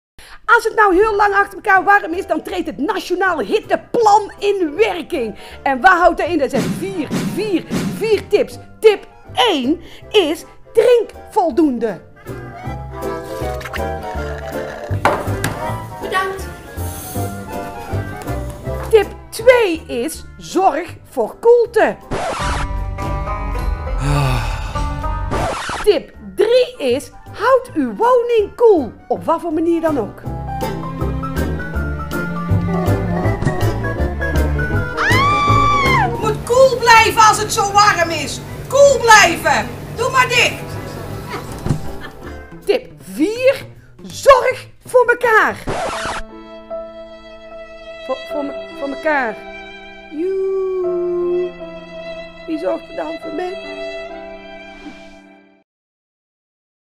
Betty legt uit hoe het Nationale Hitteplan werkt
Wat is het toch warm! Het RIVM heeft het Nationale Hitteplan in werking gesteld en Betty van den Oetelaar licht dit met liefde even toe.